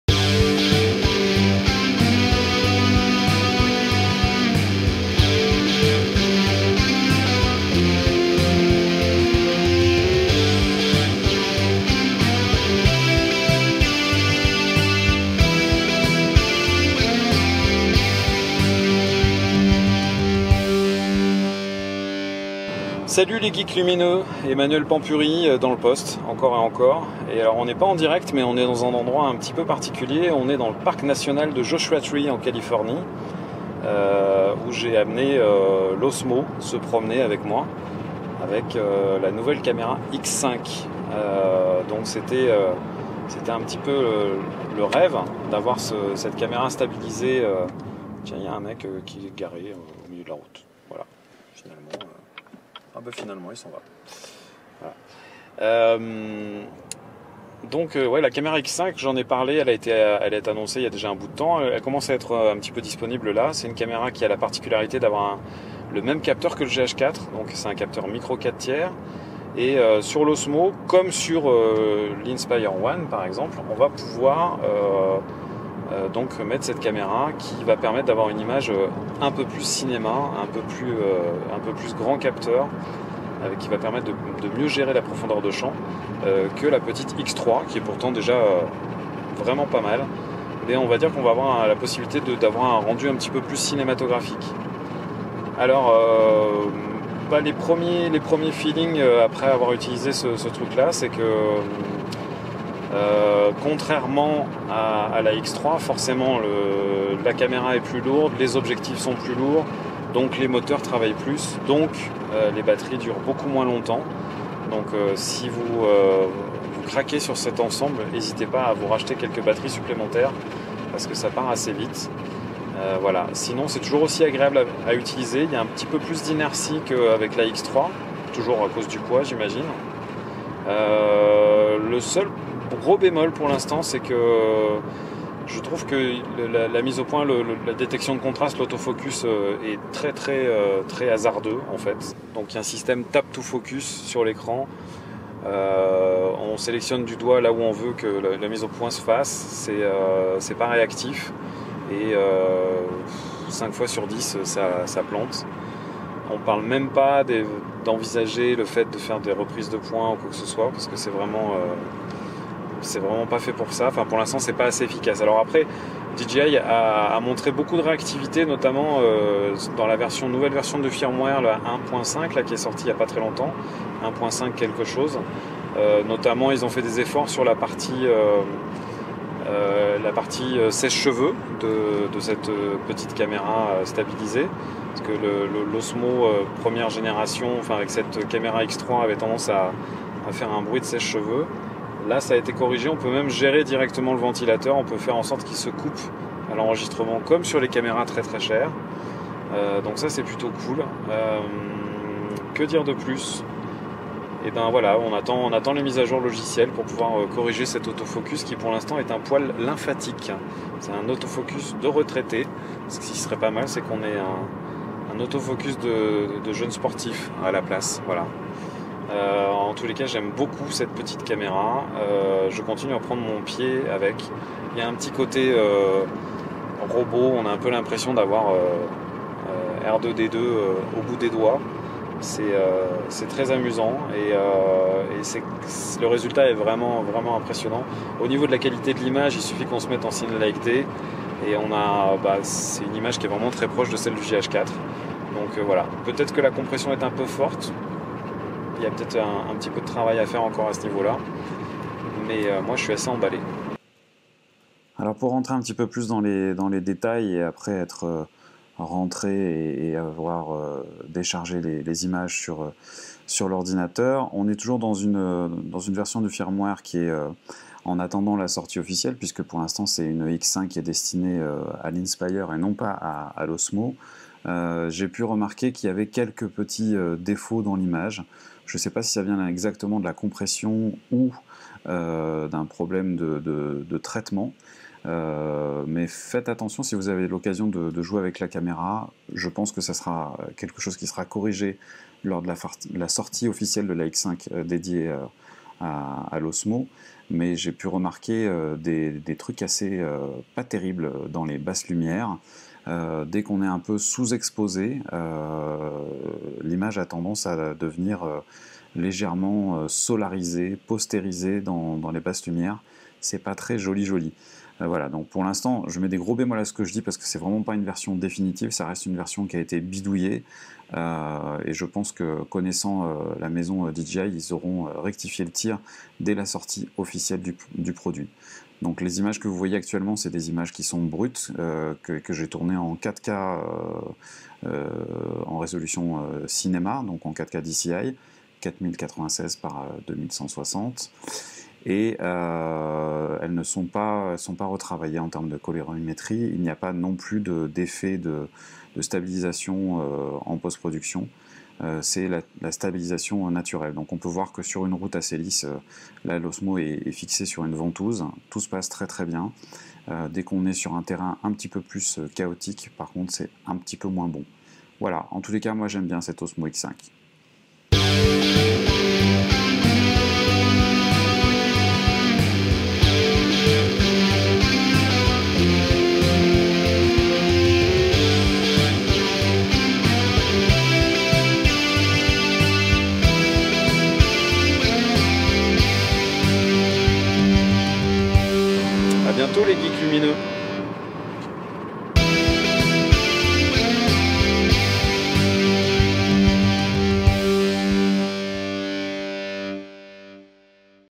DJI OSMO X5 first tests (with english subtitles)
L'osmo a fait pas mal de bruit à sa sortie, DJI avait annoncé la possibilité de changer la camera pour une version plus costaux avec une camera a capteur et monture Micro 4/3 ... premiers test alors que tout n'est pas encore finalisé ...